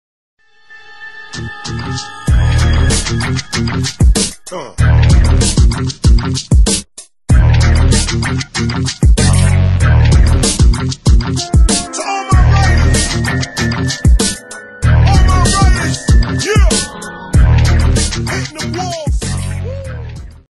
AcroSpace Barcelona
New gym AcroSpace Barcelona-Badalona
My new training spot
Breaking Breakin Break dance acro yoga parkour callisthenics tricking flipping and many more...